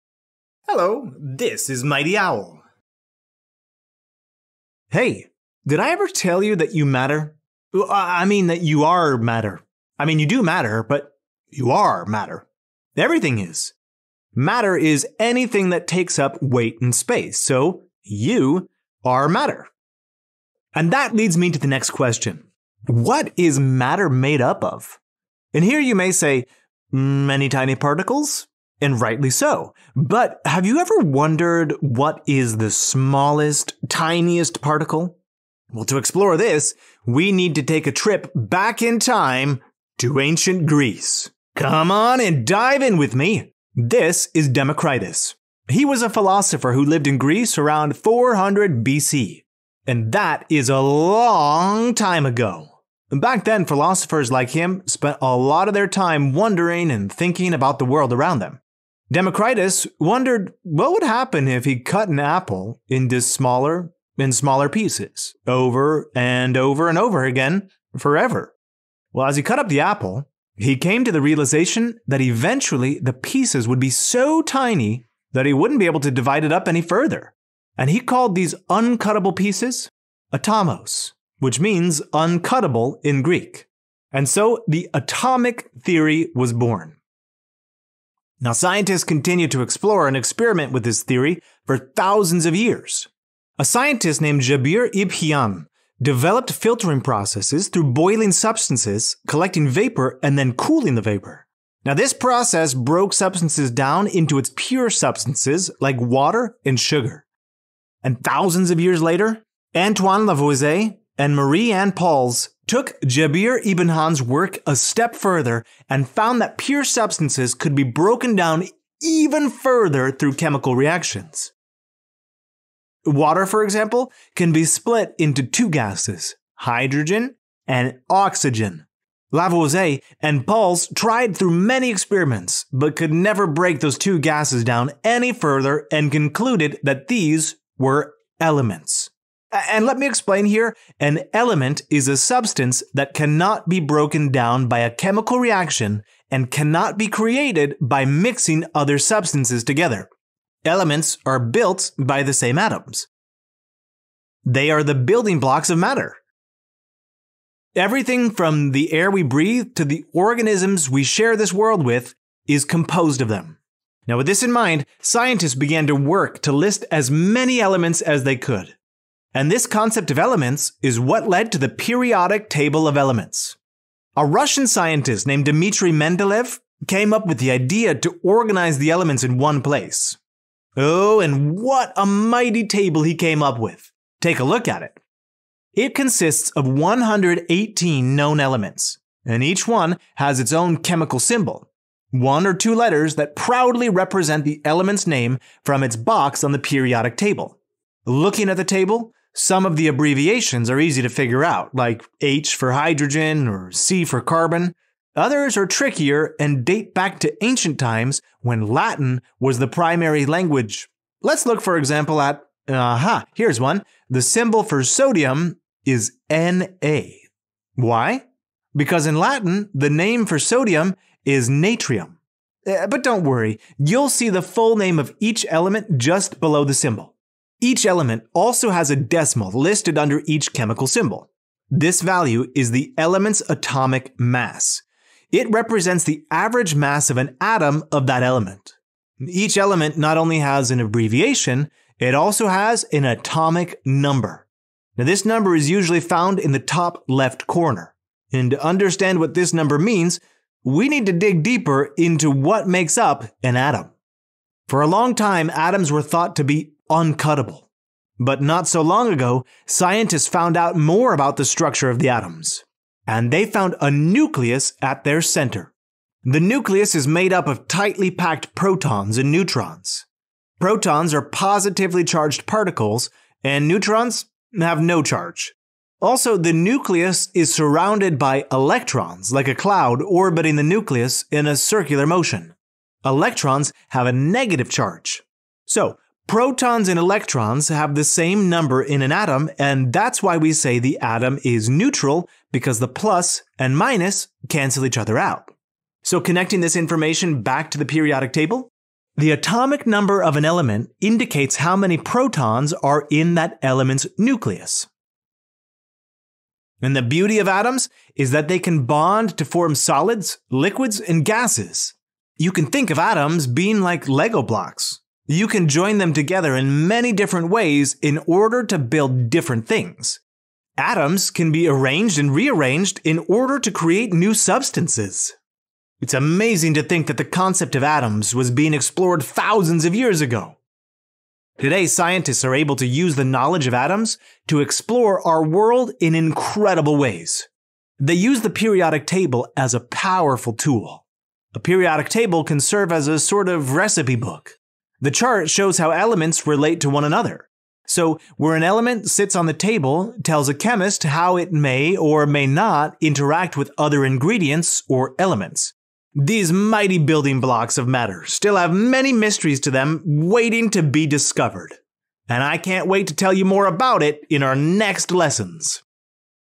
What is the smallest particle of matter? The mighty Atom! | MightyOwl Science | 5th Grade
Everything is made of matter but what is matter made of? In this video we will explore the smallest particle of matter — the mighty atom, we will learn what the atomic theory is and so much more!

Key terms: atom, atomictheory, neutrons, protons and electrons, nucleus, periodic table, element

Aligned with Next Generation Science Standards
Structure and properties of matter
5-PS1-1: Develop a model to describe that matter is made of particles too small to be seen.